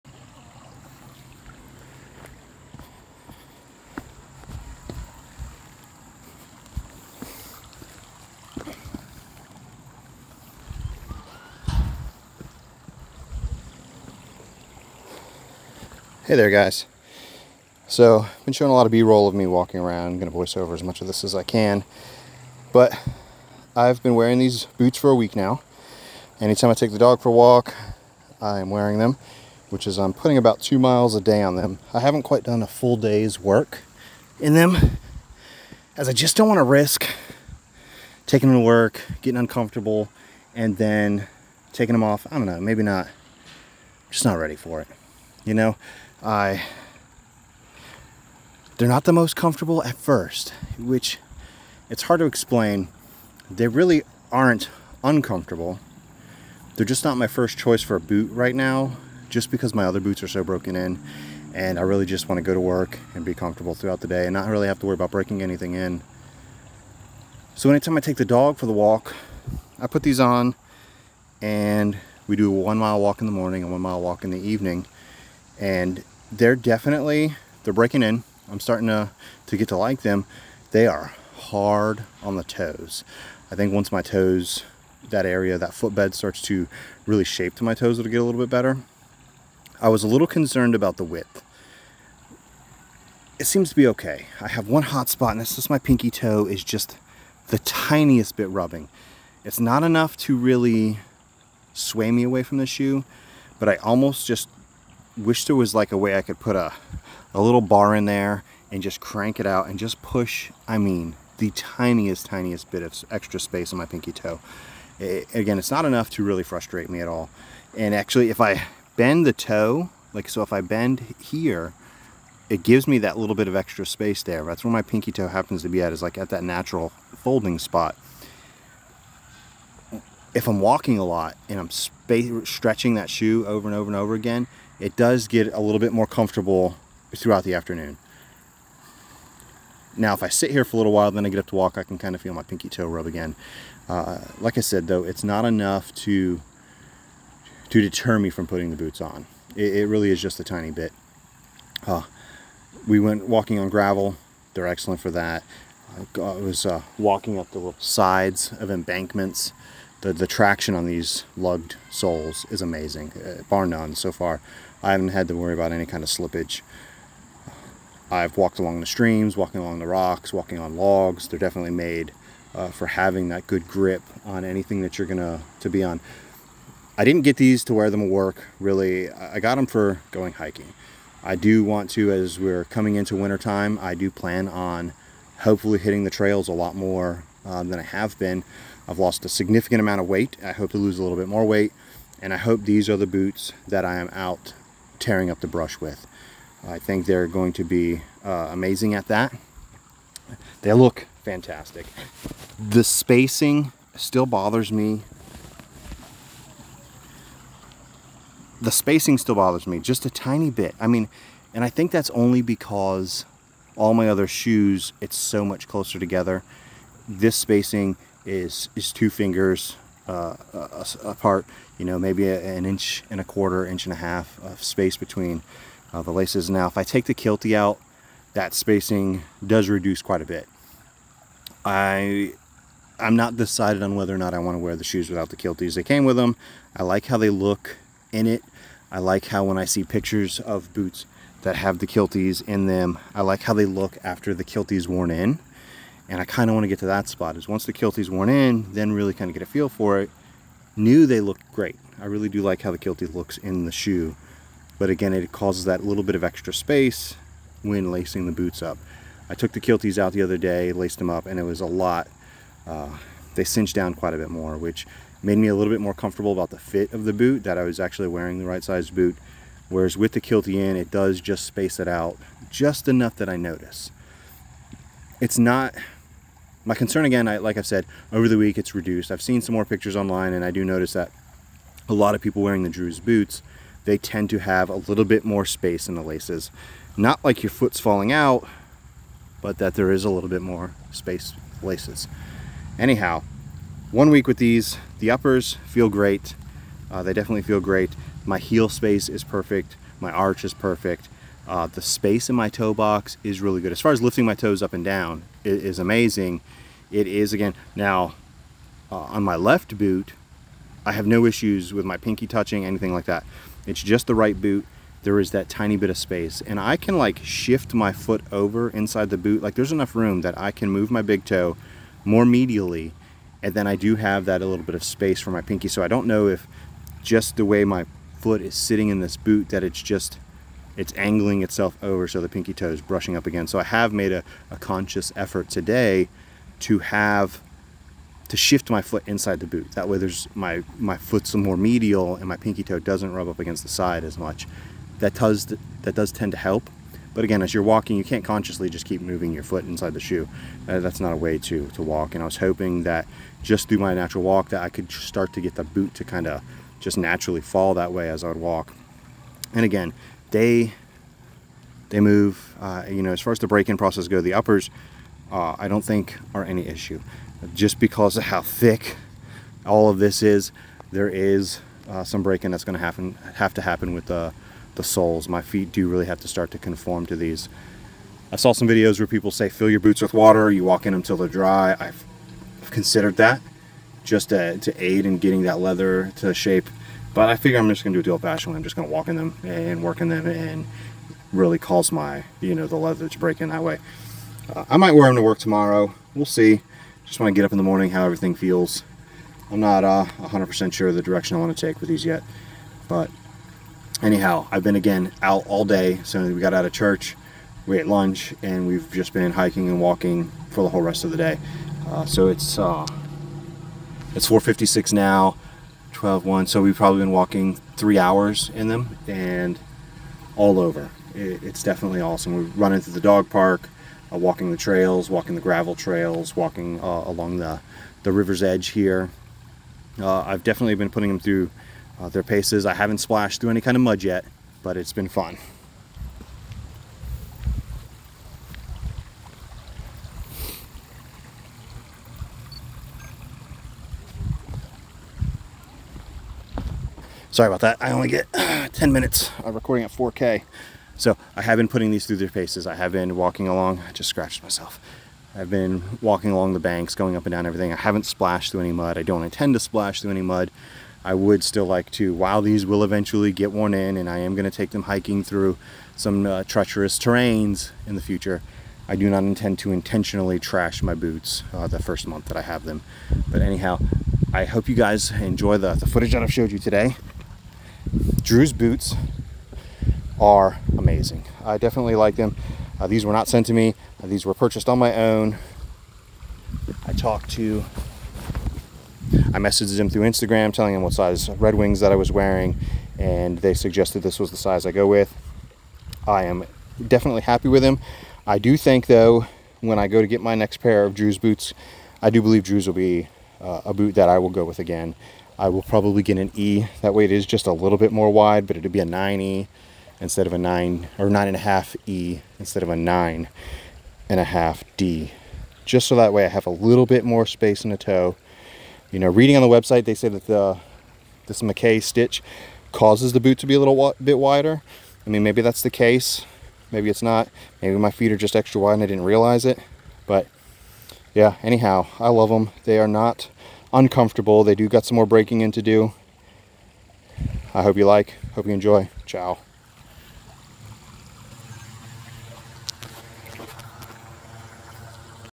Drew's Boots LTT rough-out (one-week review)
One week wearing my new Drew's boots lace to toe. These are the made-in-the-USA boot rough-out. I've been wearing them about two miles a day. While walking the puppy. On this day I wore them all day hiking and am truly satisfied with the boot. While being happy with the boot I do wish they were a tiny bit wider. On a side note, I put a wool sock over a shoe tree and jammed it deep into the toe box. Maybe a couple of days of that will loosen the width there at my pinky toe. These PNW boots are great and while the sole is still epically stiff the upper is very comfortable.